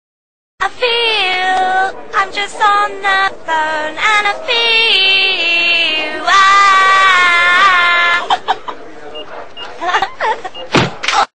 Chanter au boulot
Faut pas chanter à tue-tête au boulot cela pourrait enerver vos collègues